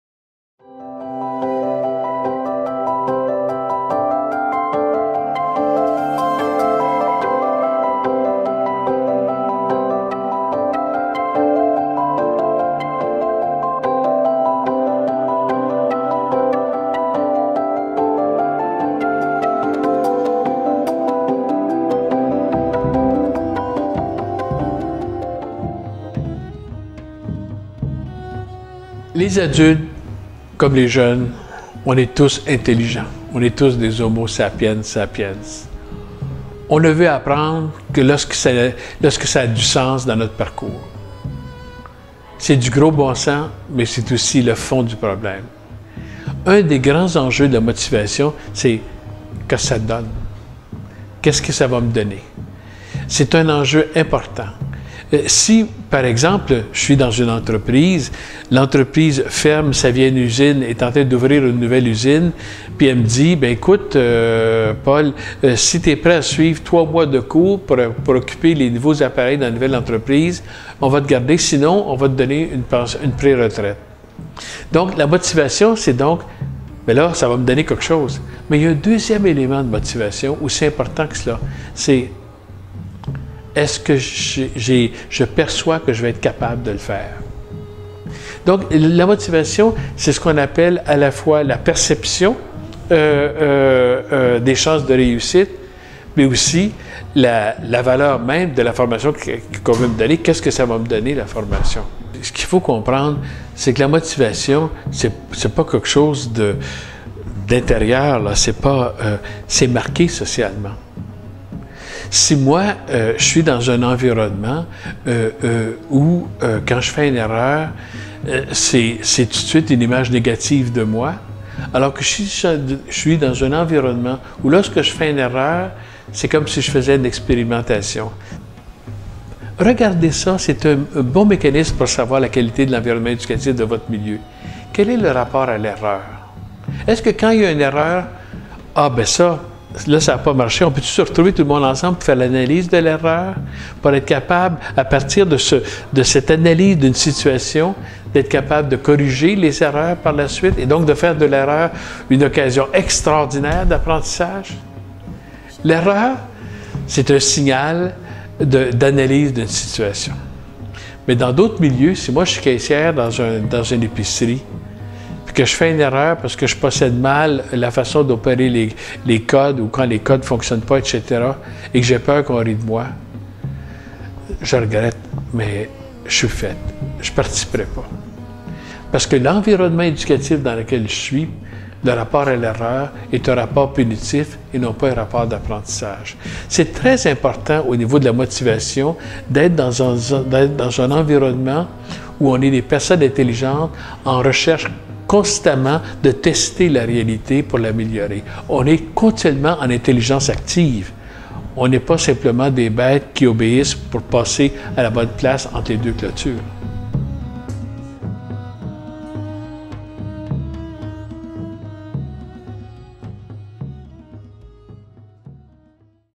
Les facteurs de motivation
Paul Bélanger est directeur du Centre interdisciplinaire de recherche/développement sur l'éducation permanente (CIRDEP) de l'UQAM.

Dans ce segment vidéo, Monsieur Bélanger explique quels sont les principaux facteurs de motivation des personnes apprenantes: apport positif et perception de réussite. De manière plus large, il aborde l'enjeu concernent l'environnement social d'apprentissage, et l'espace que ce dernier accorde au "rapport à l'erreur", à sa capacité à transformer l'erreur en situation d'apprentissage innovante.